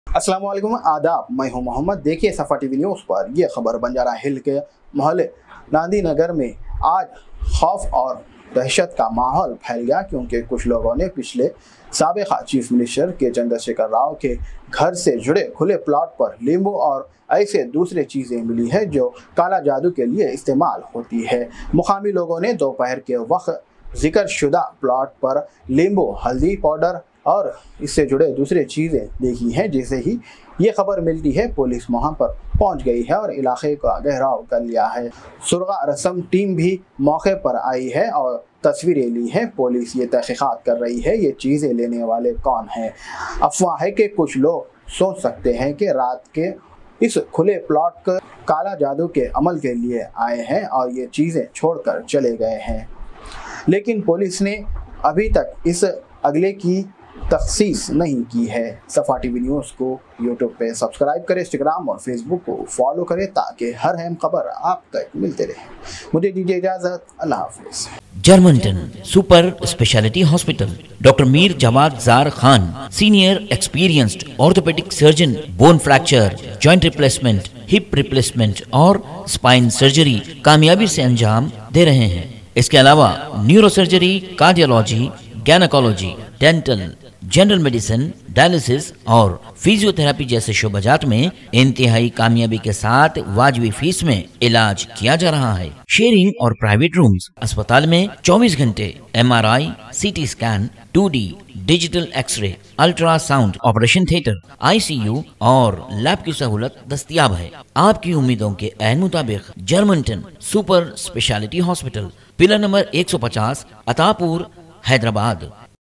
Black Magic Material Found Near KCR’s Nandi Nagar Residence at Banjara Hills
SAFA TV NEWS CHANNEL